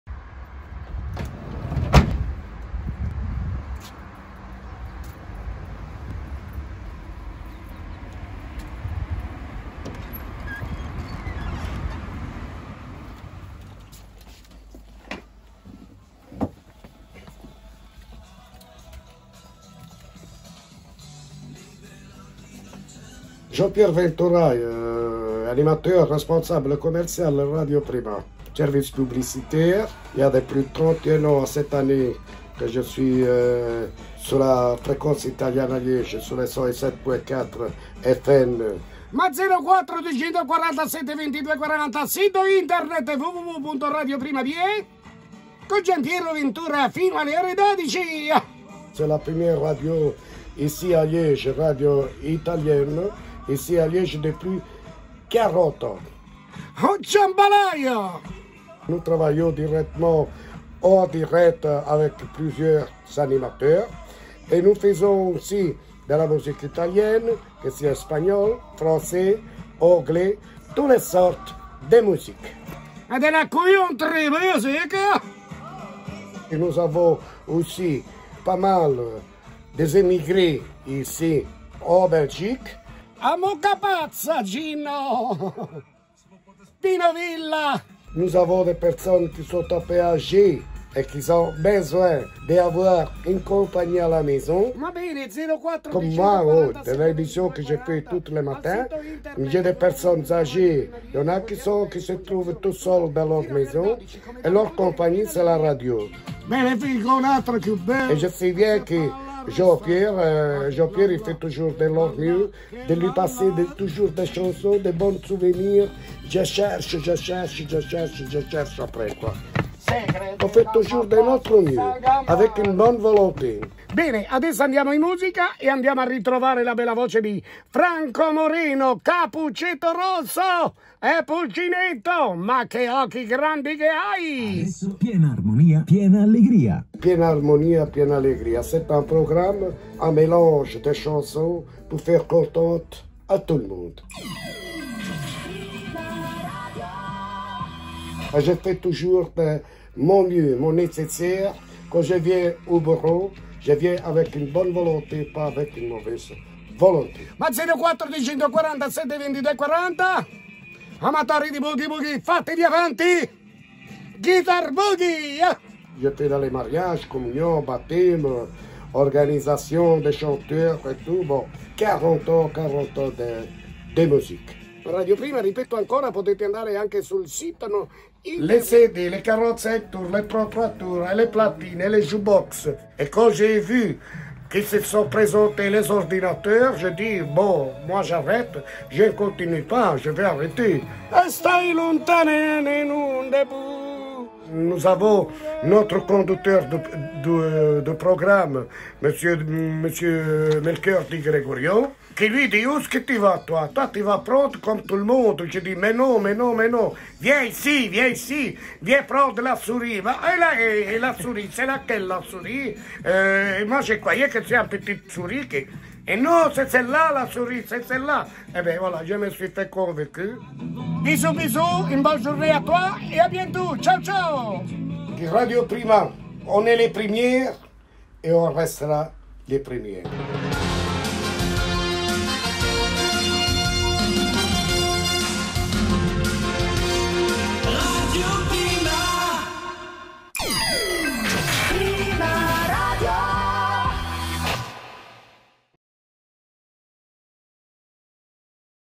Prima Story #1 - Avec Jean-Pierre Ventura
Le premier épisode de Prima Story est consacré à Jean-Pierre, animateur radio depuis de nombreuses années chez Radio Prima.

▶️ Ecoutez Radio Prima sur le 107.4 FM à Liège en Belgique, sur notre site internet et via notre application gratuite pour téléphones et tablettes 📻💻📱